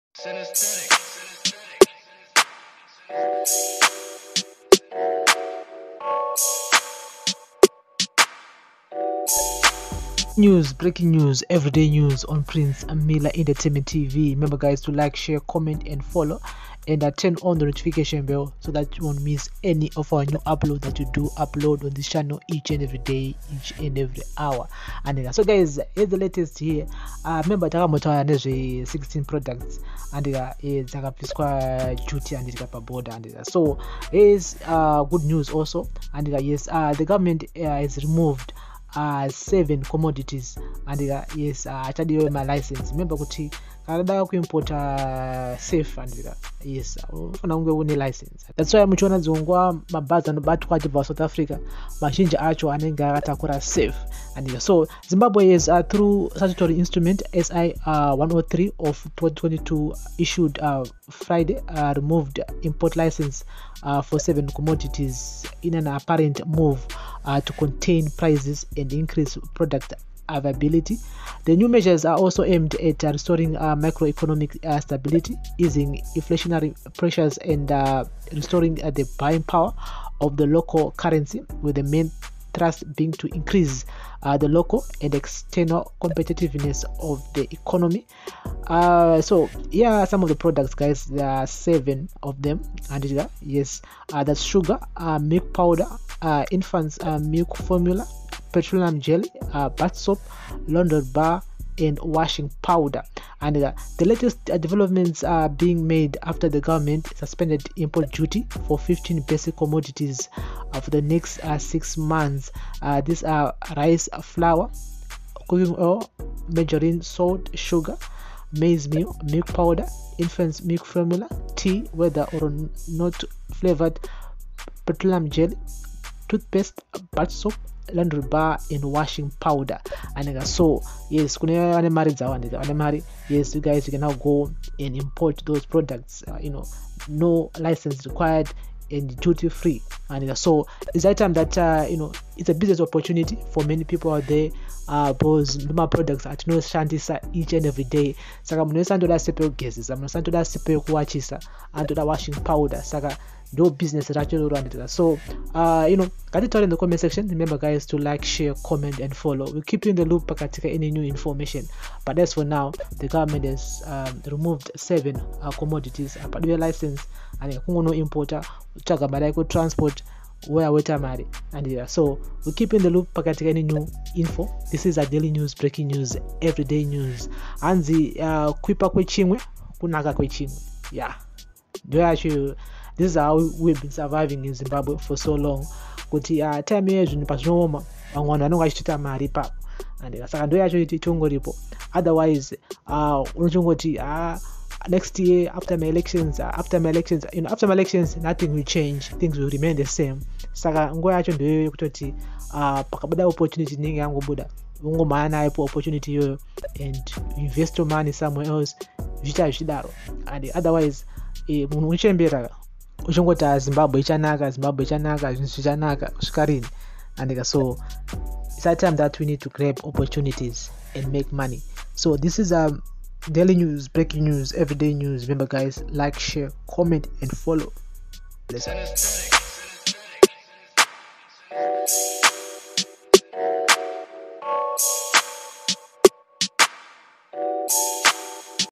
UPDATE | ZIMBABWE REMOVES IMPORT LICENCES REQUIREMENTS FOR 7 COMMODITIES | DAILY NEWS
Daily News Episode | Zimbabwe has through Statutory Instrument SI 103 of 2022 issued Friday removed inport licences for 7 common commodities in an apparent move to contain prices and increases product availability